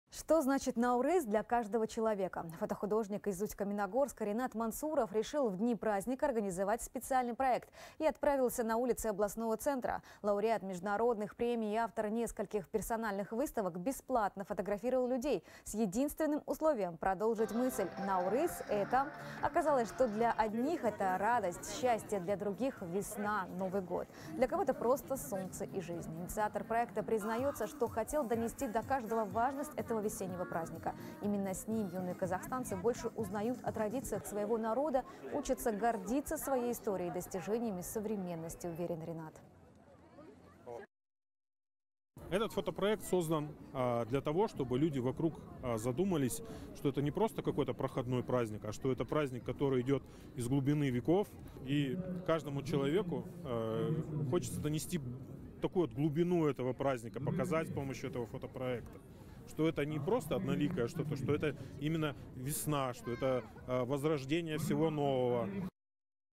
Туристический сектор Италии испытывает острую нехватку персонала
В преддверии сезона туристический сектор Италии испытывает острую нехватку персонала. Рабочих рук не хватает ресторанам и отелям. Большинство из них массово сокращали из-за простоев во время пандемии коронавируса, передаёт «Хабар 24».